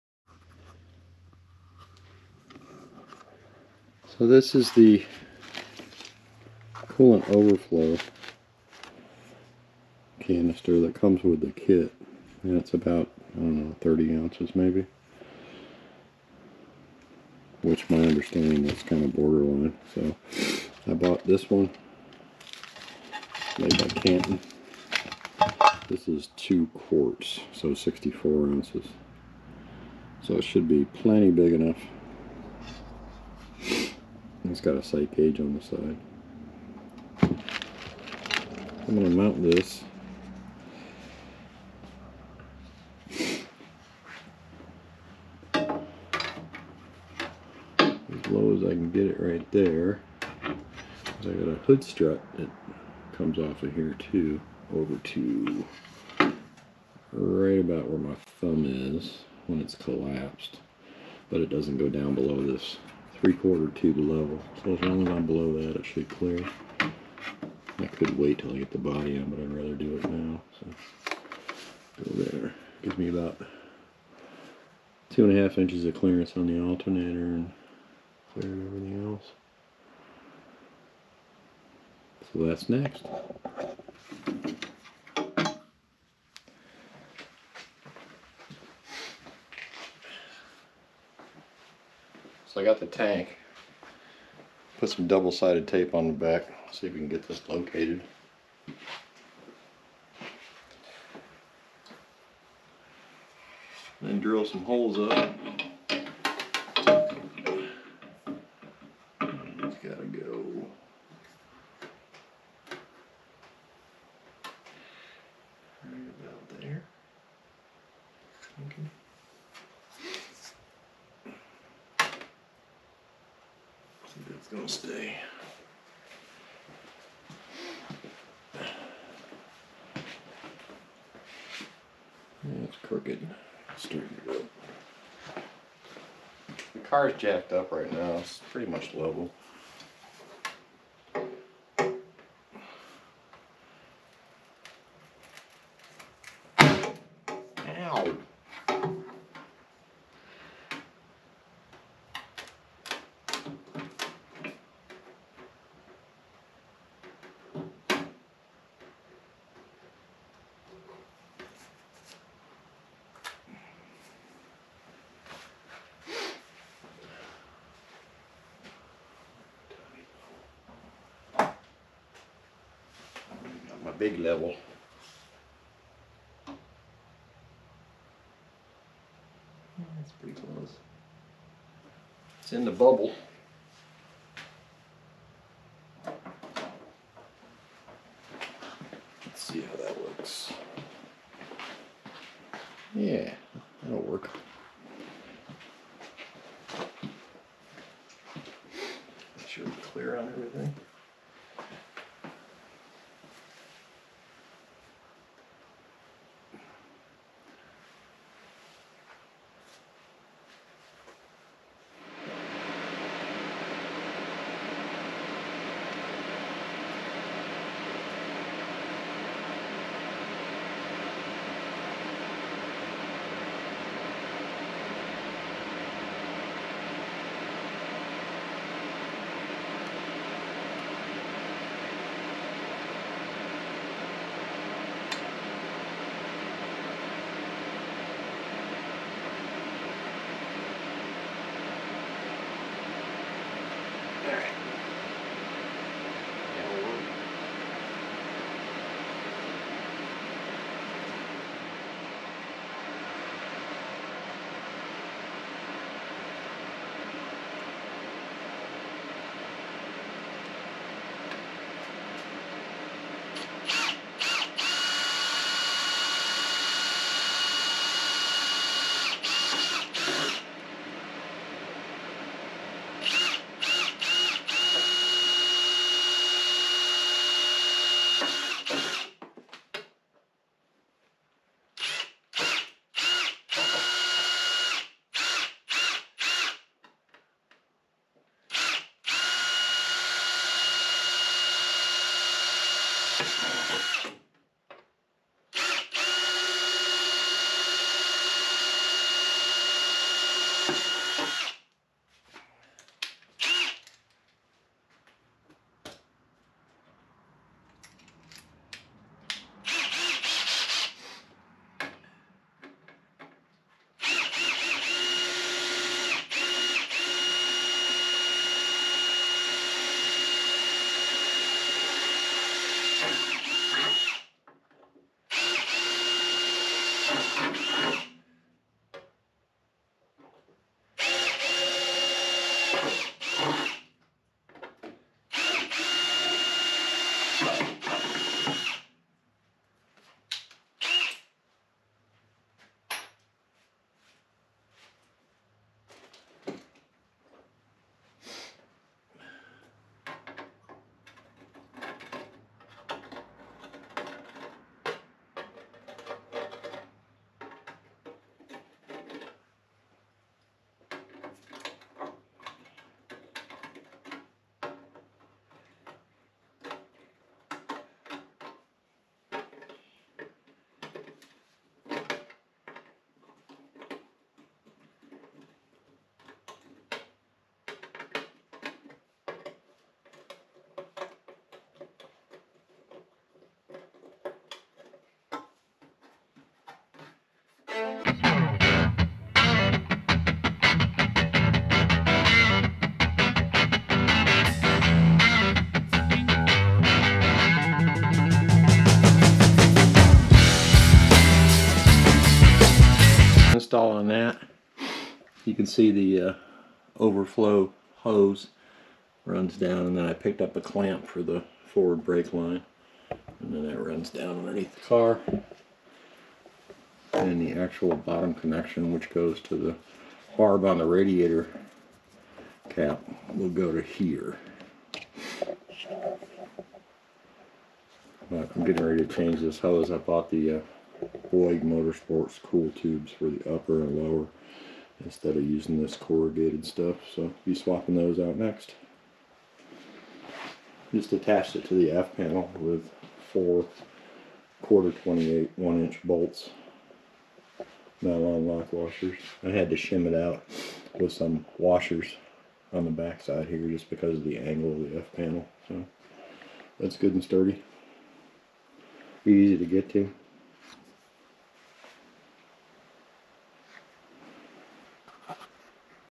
FFR MK 4 Canton coolant expansion tank installation - Shelby Cobra replica build
Higher capacity coolant expansion tank installation on passenger side F panel. Canton Racing Products 80-201 2 Quart Aluminum Recovery Tank.

Shelby Cobra replica
Factory Five Racing
FFR MK4
Factory Five Cobra
427SC 1965